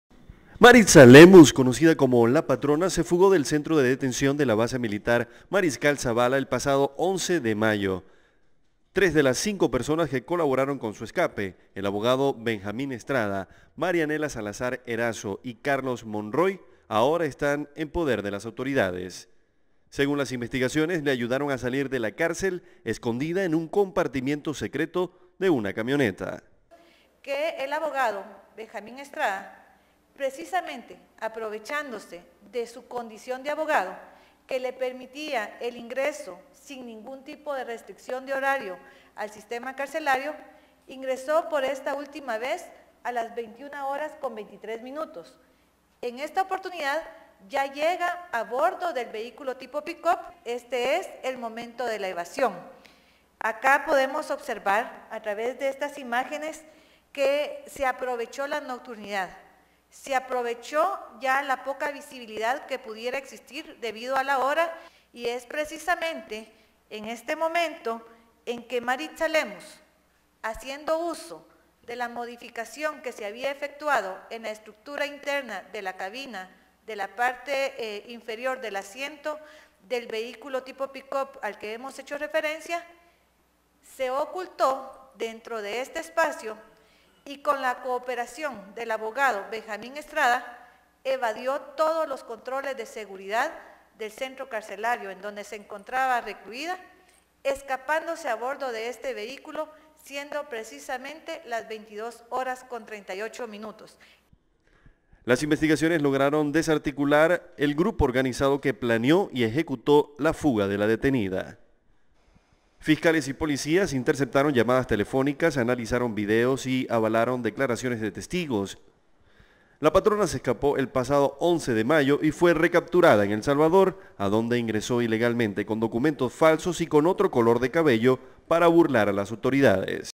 Fiscalía de Guatemala asegura que "La Patrona" usó la camioneta de su abogado cuando se fugó
En Guatemala el Ministerio Público confirmó la detención de los autores intelectuales de “La Patrona", la peligrosa líder del narcotráfico en ese país, que recientemente fue recapturada.

Maritza Lemus, conocida como “La Patrona”, se fugó del Centro de Detención de la Base Militar Mariscal Zabala el pasado 11 de mayo, tres de las 5 personas que colaboraron con su escape, el abogado Benjamín Estrada, Marielena Salazar Erazo y Carlos Monroy,  ahora están en poder de las autoridades, luego de comprobarse  que la ayudaron a salir de la cárcel escondida en un compartimiento secreto de una camioneta.